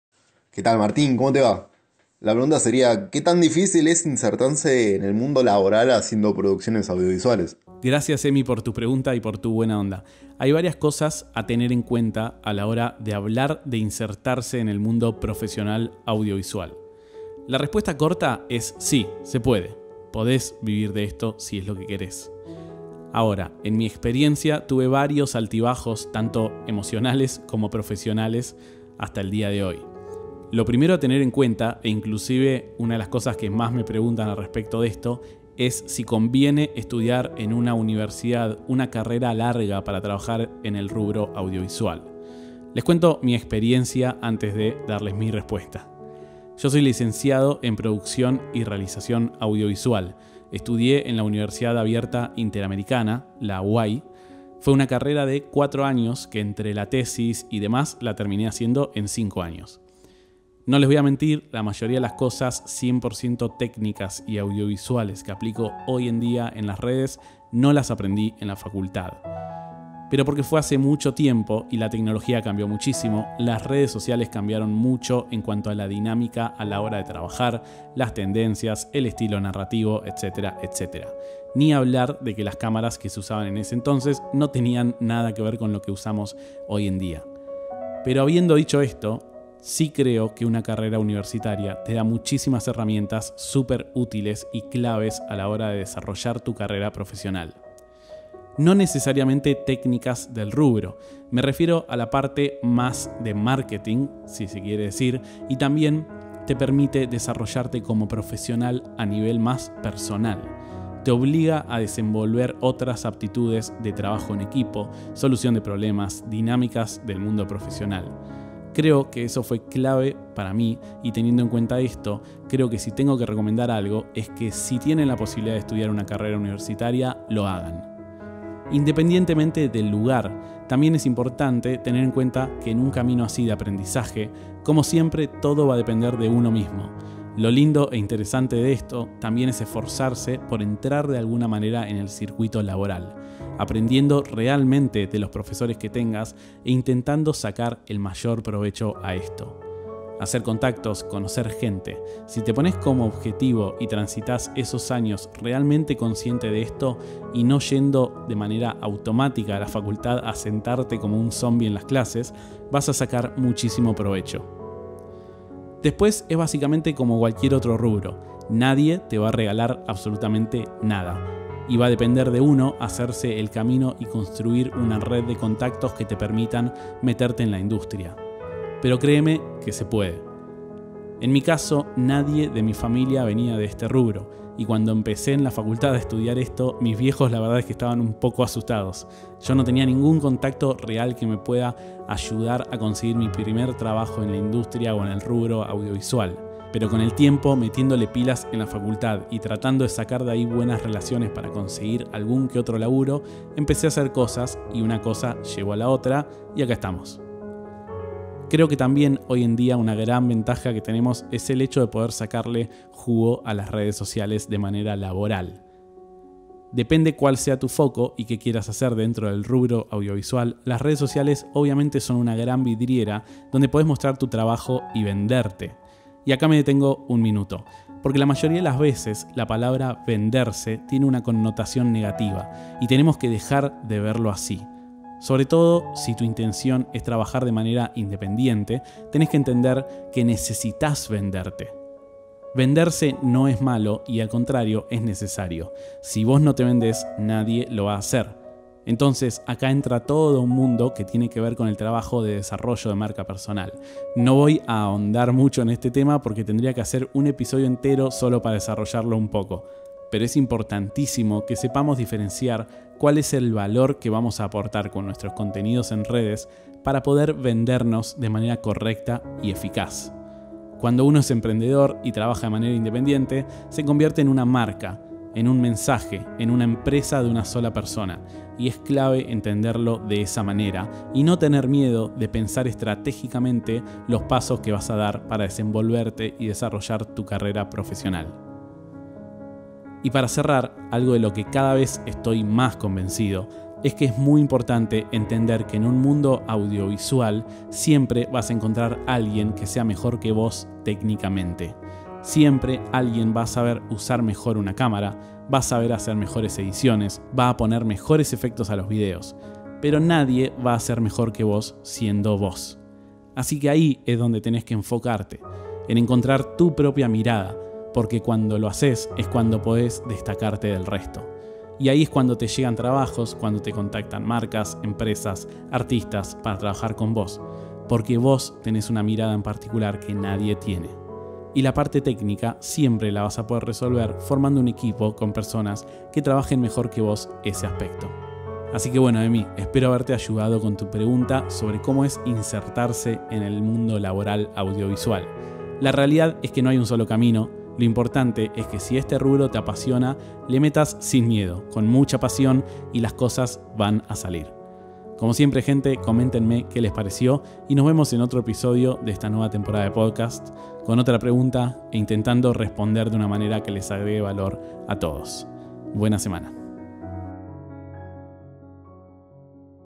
¿Cómo vivir de hacer videos? Podcast // Temporada 2 - EP03
Nueva temporada de los Podcast. Esta vez, contestando audios que me
llegan a mi cuenta de instagram @martindeiuliis